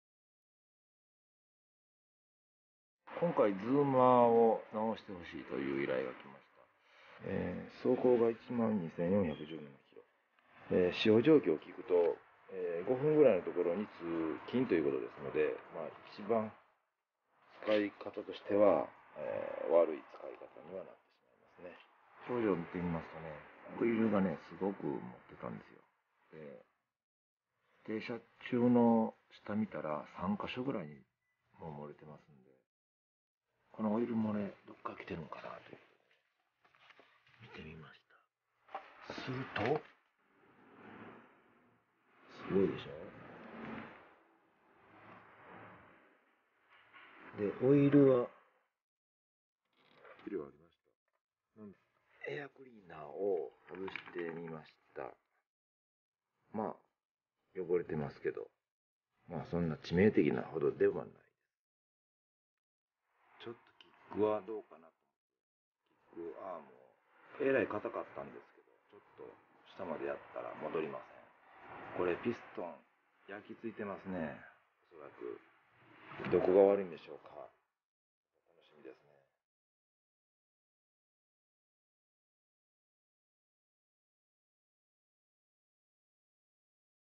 乗りっぱなしで壊れたズーマーを修理します踏板車維修視頻
一度もメンテナンスをすること無く乗りっぱなしでいきなりエンジンから煙が出て、止まってしまったようですどこがどうなっているのでしょうか？分解整備しますが、、、ここまで悪いとは思わなかったです#ZOOMER＃スクーター＃整備